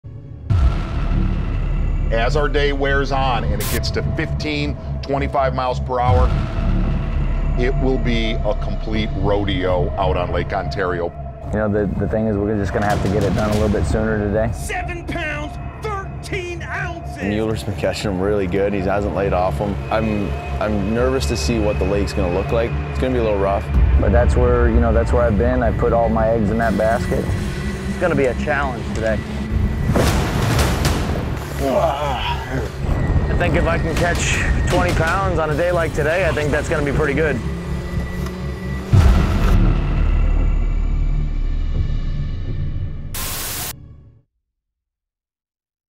The Battle on Ontario (St. Lawrence River)
Tomorrow (August 9th)! Tune in at 6 pm ET for the Battle on Ontario between Chris Johnston and @Paul Mueller. Wind, waves, big water and even bigger smallmouth. Watch the back-and-forth action between these two stellar smallmouth anglers from the Final Day of the St. Lawrence River. Plenty of storylines and drama surrounded the two prominent figures of this event. Available on all social platforms and Bassmaster.com.